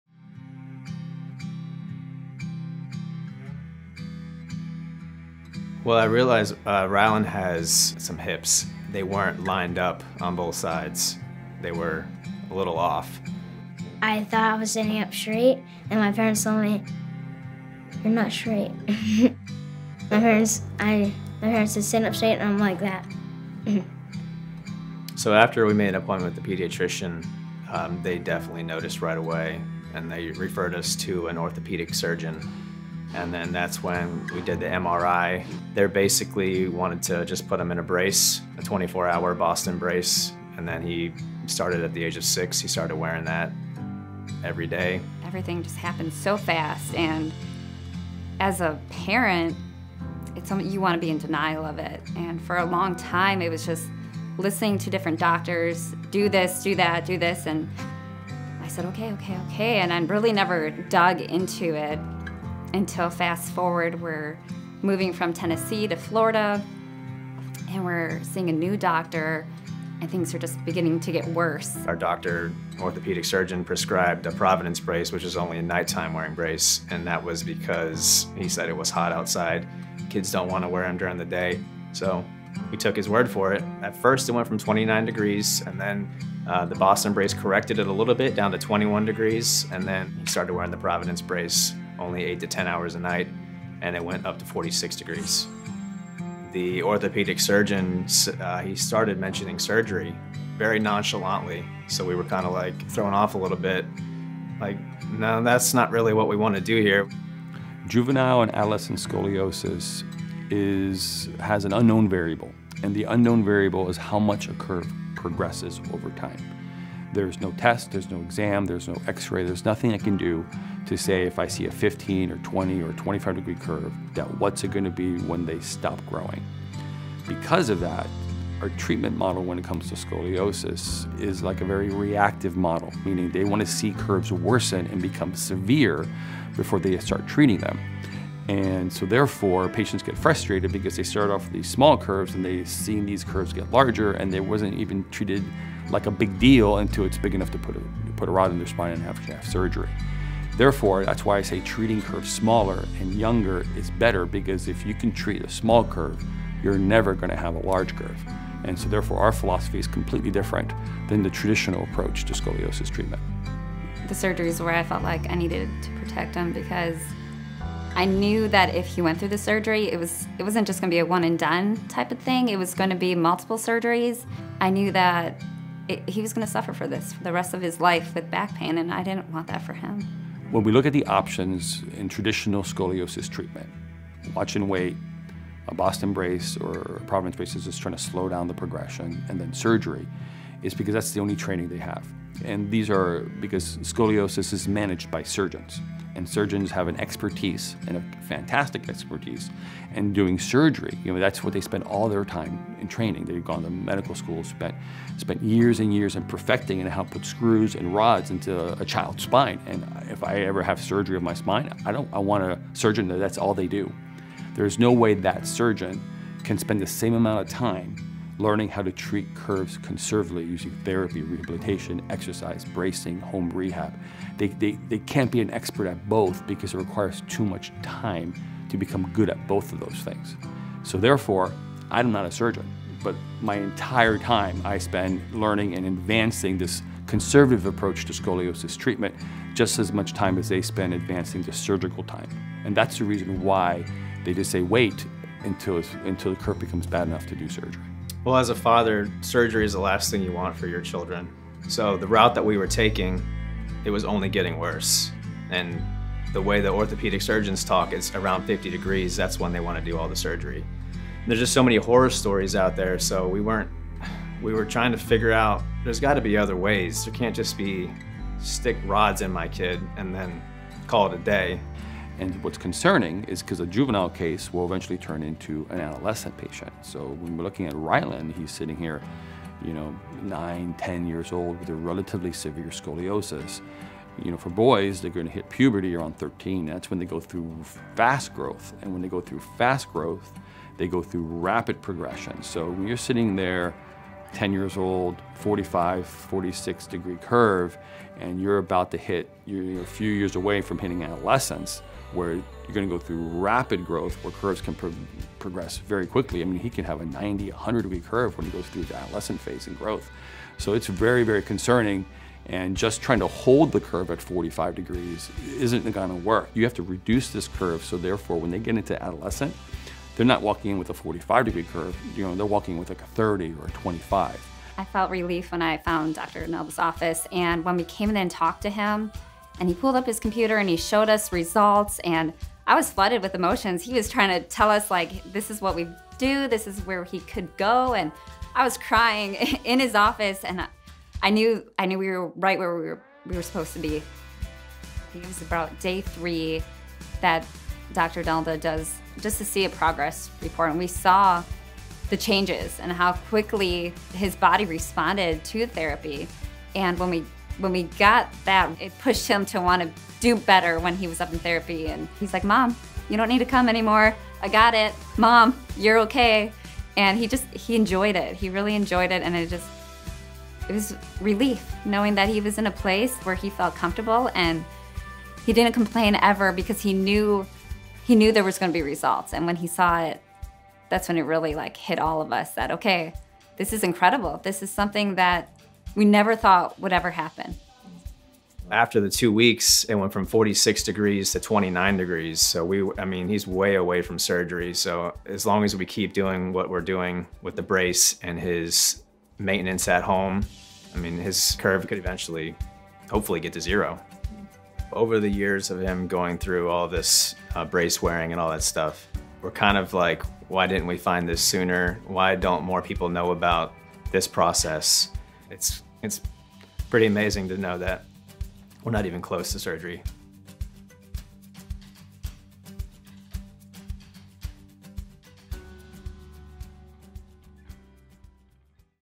Adoloscent Scoliosis Progression & Treatment Success: Rylen's Story
Rylen was only six years old when his parents noticed that his hips did not align properly and that he could not stand up straight. Rylen thought that his standing posture was not strange because he was used to the misalignment in his spine.

After noticing this issue, Rylen was examined by his pediatrician and referred to an orthopedic surgeon. This doctor confirmed that Rylen has scoliosis and prescribed a Boston brace to be worn 24 hours a day.

This Boston brace helped Rylen's curve reduce from 29 degrees to 21 degrees.

After moving to another state, a different doctor prescribed a Providence brace for nighttime use only because of the hot temperatures in the city during the day. This brace made Rylen's scoliosis worse, causing it to go from 21 degrees to 26 degrees. After analyzing the progression, their orthopedic surgeon suggested scoliosis surgery.

Rylen's parents found and visited Dr. Nalda at the Scoliosis Reduction Center in search of another perspective. After consultation, they agreed to start treatment at the Center after seeing that Rylen's scoliosis could be effectively reduced while maintaining and improving spine function.

Rylen's previous doctors made the mistake of not considering how the condition could rapidly progress once he hits puberty. During this period, adolescents go through growth spurs that could trigger scoliosis to become worse. The doctors took a reactive perspective to Rylen's case by watching and waiting for his spine to get worse, then suggesting surgery.

After treating Rylen for two weeks, his curve reduced from 46 degrees to 29 degrees using various tools from a tailored, conservative treatment approach.

These results showed Rylen and his family that his condition wouldn't be putting any limits in his life, giving him a boost of confidence to keep working with Dr. Nalda at the Scoliosis Reduction Center.

00:00 - Rylen's Patient Story
00:25 - Initial Diagnosis & Treatment Suggestions
01:00 - Providence Brace And Its Consequences
01:47 - Risk Factor In Juvenile Scoliosis Cases
02:43 - Scoliosis Surgery Risks
03:00 - Traditional Scoliosis Treatment Approach
04:32 - Rapid Growth Periods
05:25 - Finding Hope
05:51 - Rylen's Results